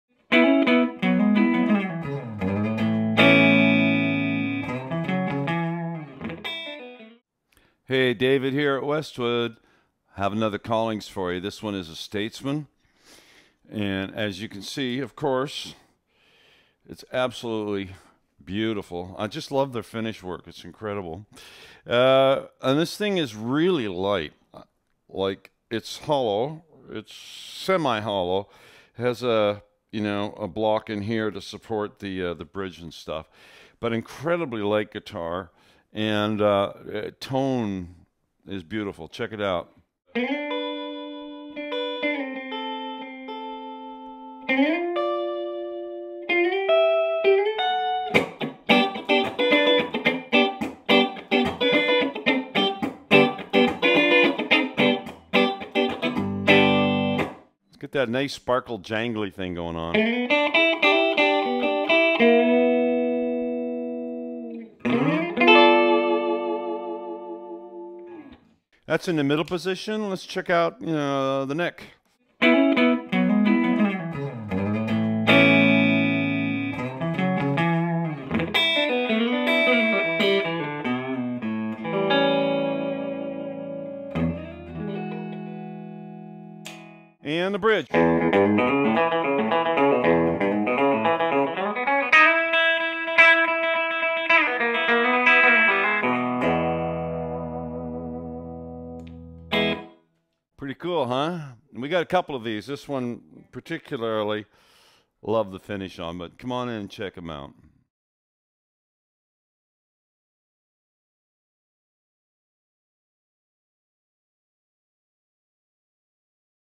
Collings Statesman LC Trans Orange
David demos this beautiful new Collings States LC in Trans Orange, available on our Reverb Shop